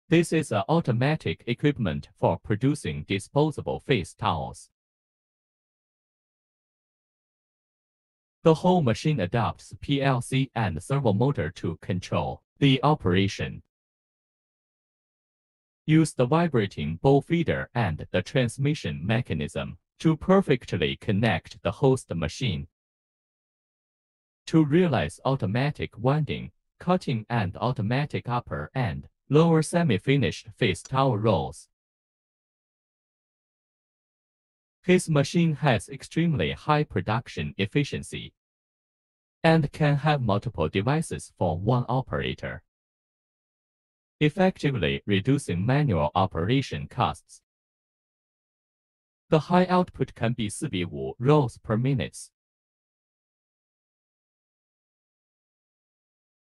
Cotton Soft Towel Rewinding Machine | Why do we use cotton soft towels
This is an automatic cotton soft towel rewinding machine. Disposable face towels can not only be used to wash face, but also can be used to remove makeup, makeup, lotion, etc. The whole machine adopts PLC and servo motor to control the operation, Use the vibrating bowl feeder and the transmission mechanism to perfectly connect the host machine to realize automatic winding + cutting + automatic upper and lower semi-finished face towel rolls. The output can be 4-5 rolls /min.